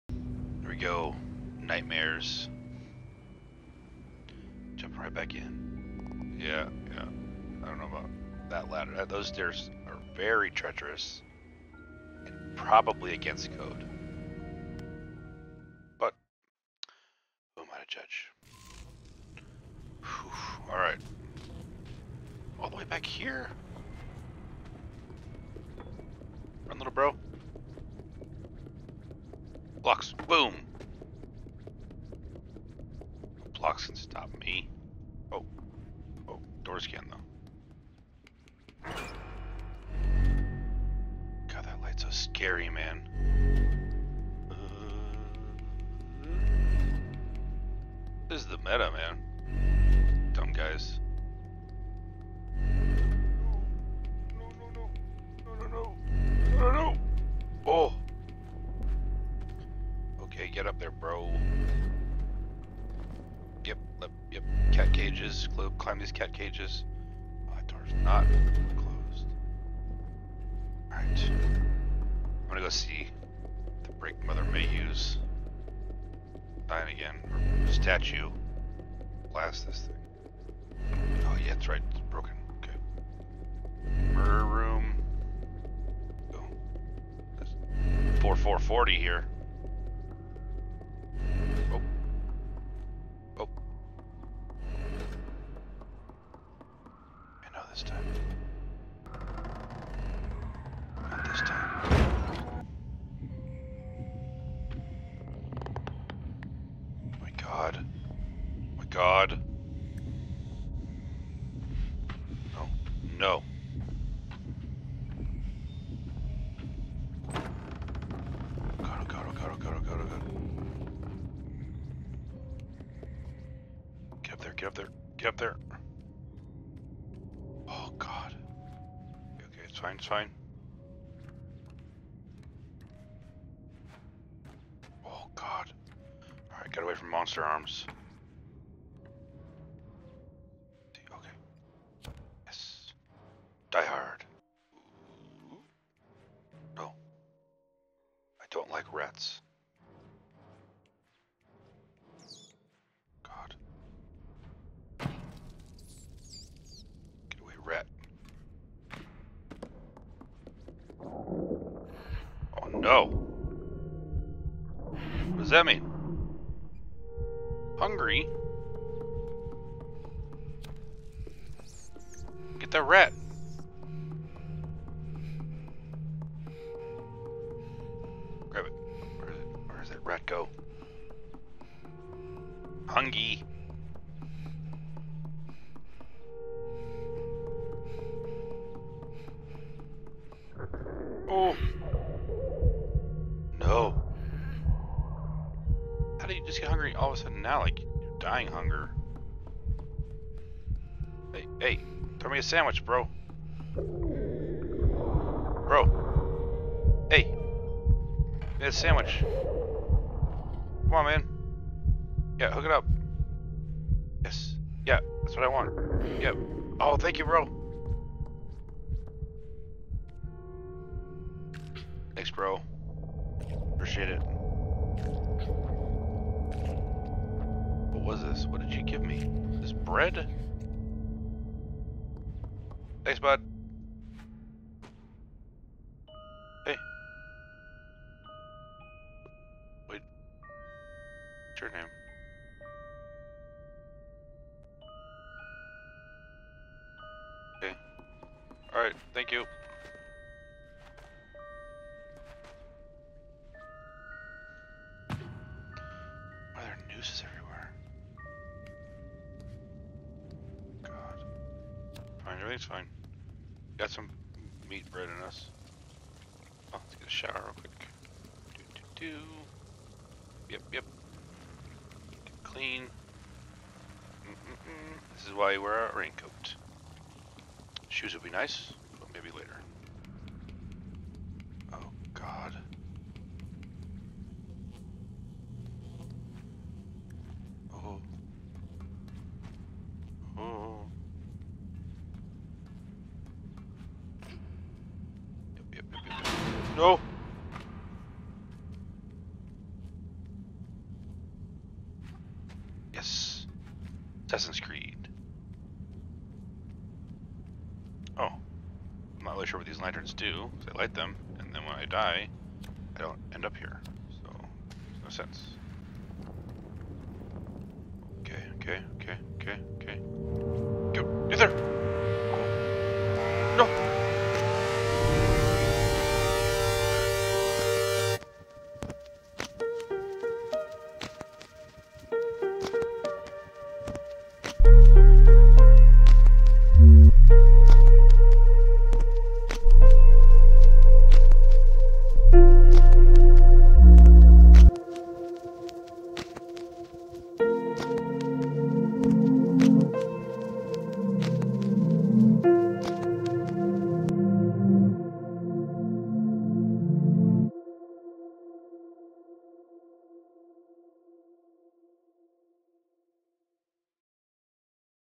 The Yellow Jacket Gang Electric Boogaloo
We are past the eye but there are still some sludge leeches.  The Yellow Jacket Gang doesnt know fear yet.  We are just getting started.

Welcome to 5ish min games with Tick.